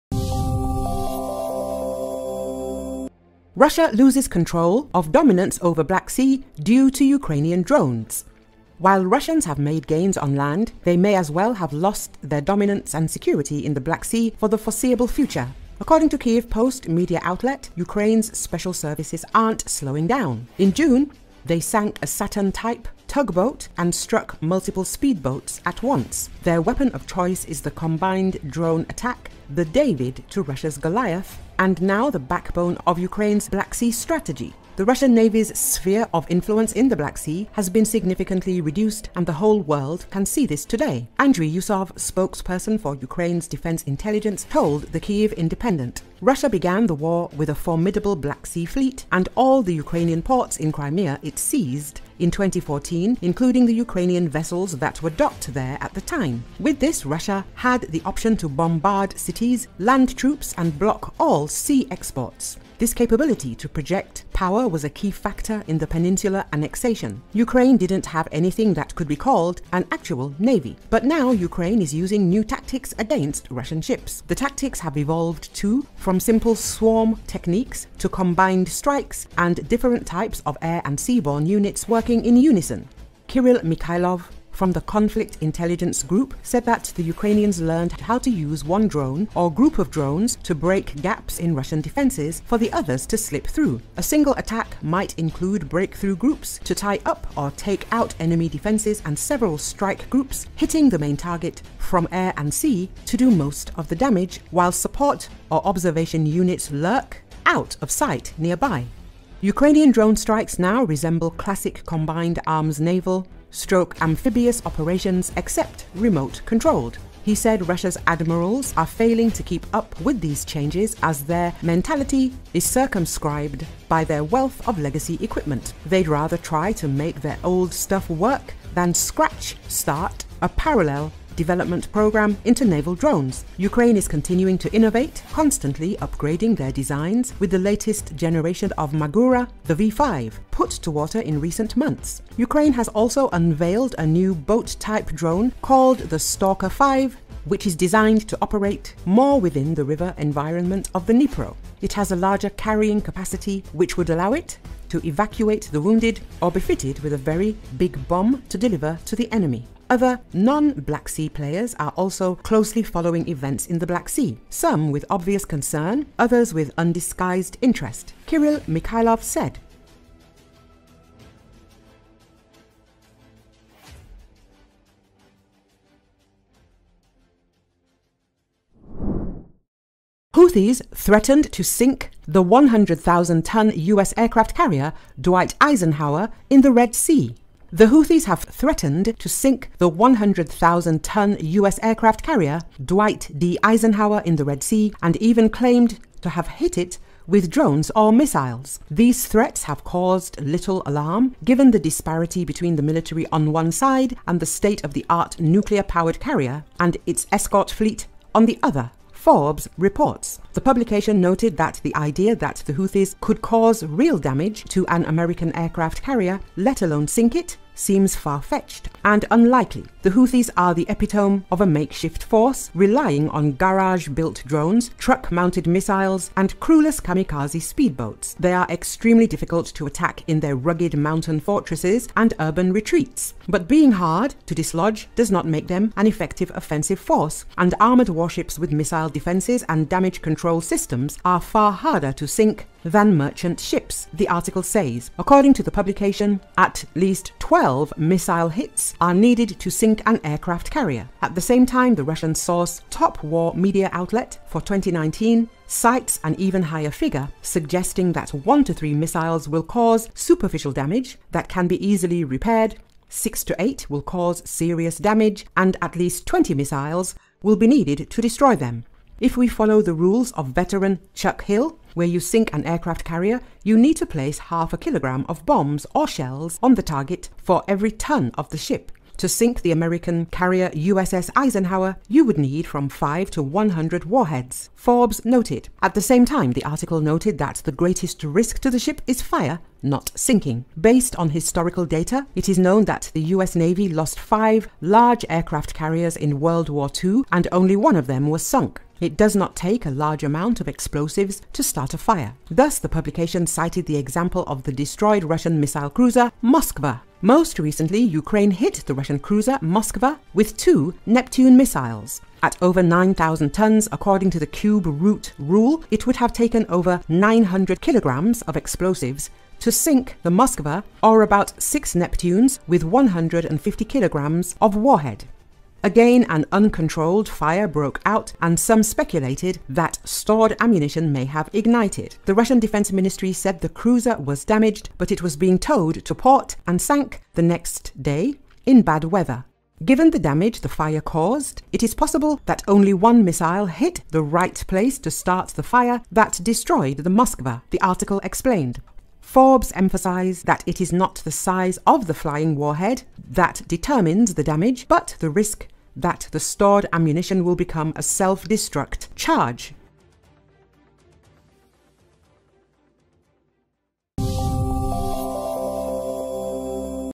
Russia loses control of dominance over Black Sea due to Ukrainian drones
While Russians have made gains on land, they may as well have lost their dominance and security in the Black Sea for the foreseeable future.  According to Kyiv Post media outlet, Ukraine’s special services aren’t slowing down. In June, they sank a Saturn-type tugboat and struck multiple speedboats at once. Their weapon of choice is the combined drone attack — the David to Russia’s Goliath, and now the backbone of Ukraine’s Black Sea strategy.
“The Russian Navy’s sphere of influence in the Black Sea has been significantly reduced, and the whole world can see this today,” Andrii Yusov, spokesperson for Ukraine’s Defense Intelligence told the Kyiv Independent.
Russia began the war with a formidable Black Sea fleet and all the Ukrainian ports in Crimea it seized in 2014, including the Ukrainian vessels that were docked there at the time. With this, Russia had the option to bombard cities, land troops, and block all sea exports. This capability to project power was a key factor in the peninsular annexation. Ukraine didn’t have anything that could be called an actual navy. But now Ukraine is using new tactics against Russian ships.
The tactics have evolved too, from simple swarm techniques to combined strikes with different types of air and sea-borne units working in unison.
Kirill Mikhailov from the Conflict Intelligence Group said that the Ukrainians learned how to use one drone or group of drones to break gaps in Russian defenses, for the others to slip through.
A single attack might include breakthrough groups to tie up or take out enemy defenses and several strike groups hitting the main target from air and sea to do most of the damage, while support or observation units lurk out of sight nearby. Ukrainian drone strikes now resemble classic combined arms naval/amphibious operations, except remote-controlled.
He said Russia’s admirals are failing to keep up with these changes as their mentality is circumscribed by their wealth of legacy equipment. They’d rather try to make their old stuff work than scratch start a parallel development program into naval drones.
Ukraine is continuing to innovate, constantly upgrading these designs, with the latest generation of Magura, the V5, put to water in recent months. Ukraine has also unveiled a new boat-type drone called the Stalker 5, which is designed to operate more within the river environment of the Dnipro. It has a larger carrying capacity, which would allow it to evacuate the wounded, or be fitted with a very big bomb to deliver to the enemy.
“Other, non-Black Sea players are also closely following events in the Black Sea. Some with obvious concern, others with undisguised interest.”, Kirill Mikhailov said.